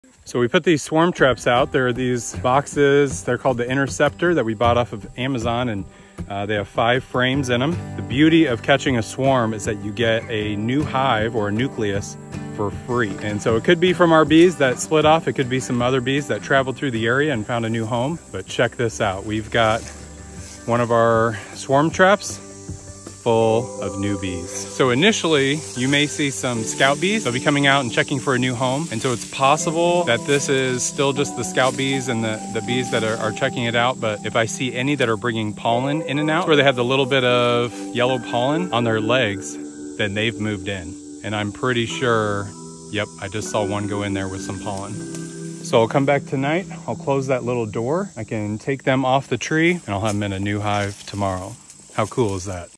We caught wild bees!
Look what we caught in our swarm trap...

The hive we used